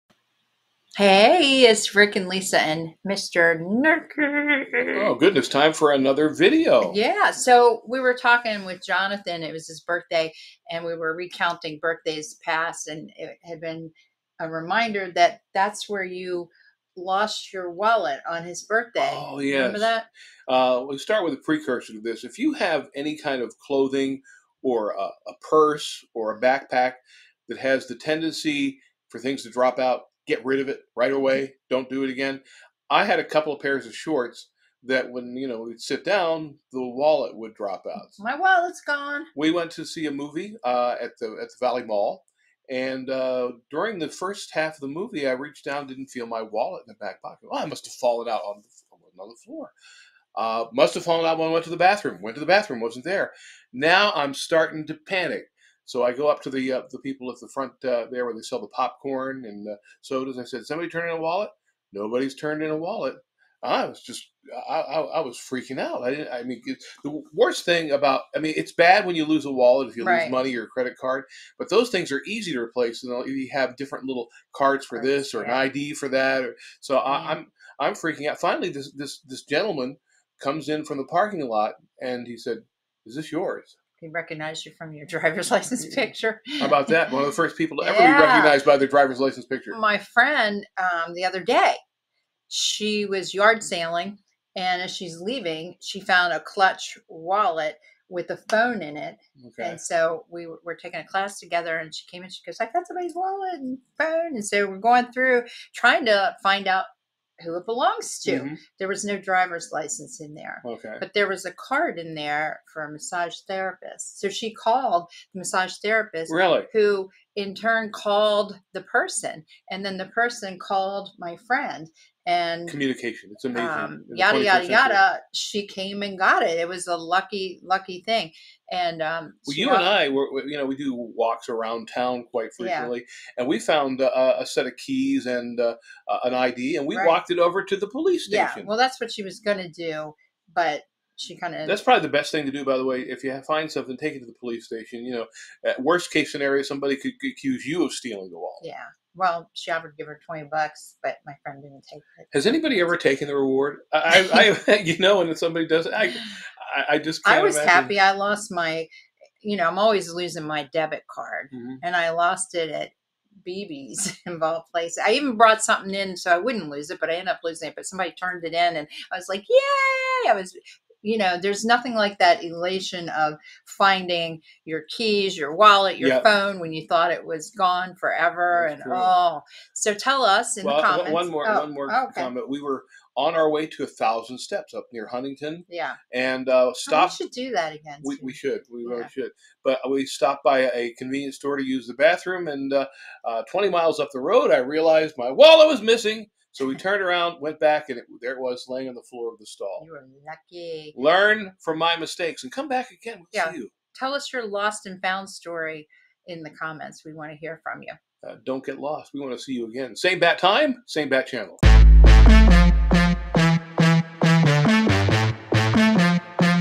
Finding and Losing Stuff
Rick lost his wallet and my friend found one!